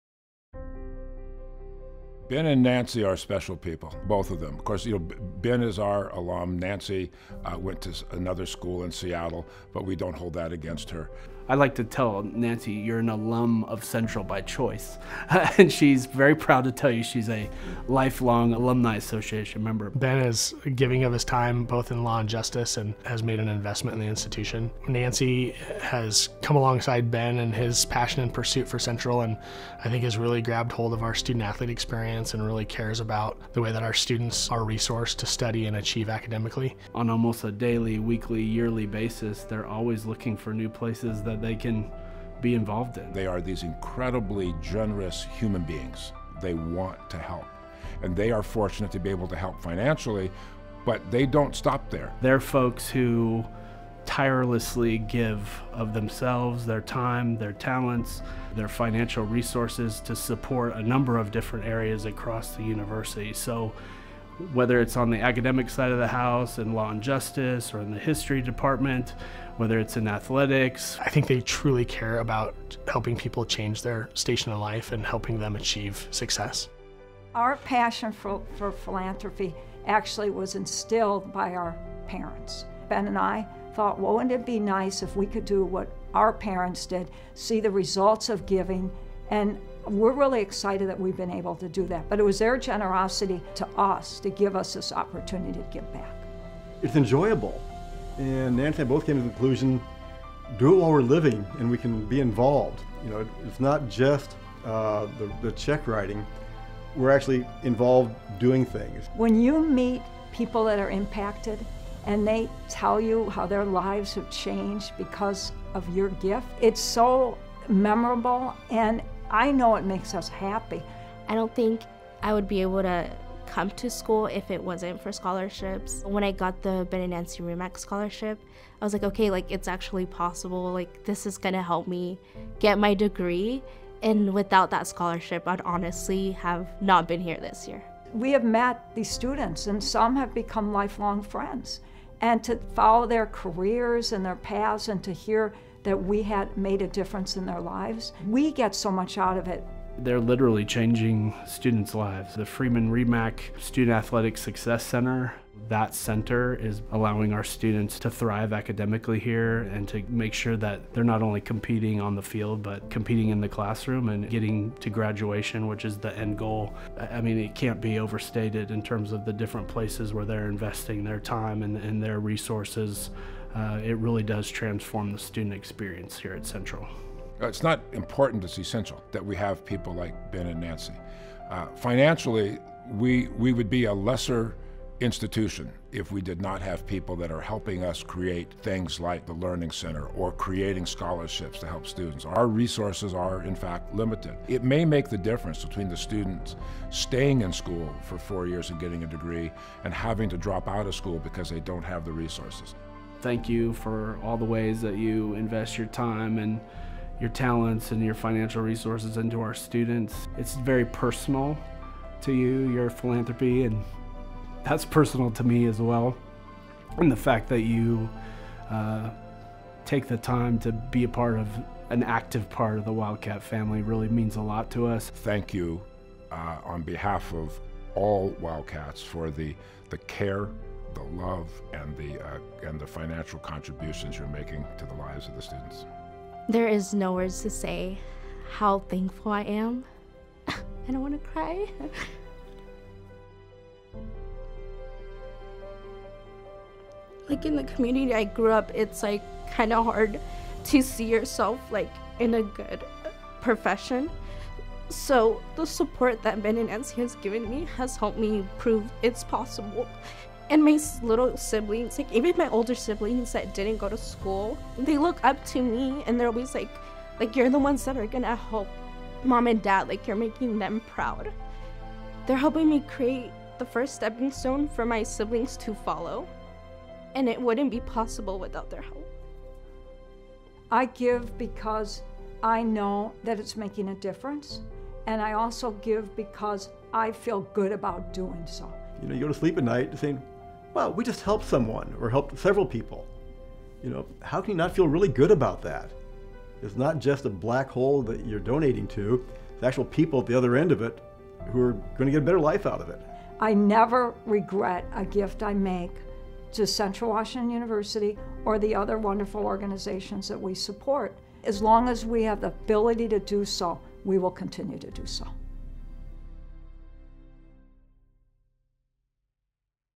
2019 CWU Philanthropists of the Year
Ben and Nancy Remak were named Philanthropists of the Year at the 2019 Distinguished Alumni Awards, put on by the Central Washington University Alumni Association.

Ben and Nancy invest their time, talents, and financial resources across the CWU campus. In addition to providing full-tuition scholarships in law and justice and history, they also financially support CWU volleyball, football, the Wildcat Commons, and the Freedman-Remak Student-Athlete Academic Success Center. Ben currently serves on the CWU Law and Justice Department Advisory Board while Nancy has recently joined the College of Arts and Humanities Advisory Board.

Thank you Ben and Nancy for all that you do to support CWU and its current and future students.

Video by Creative Asset.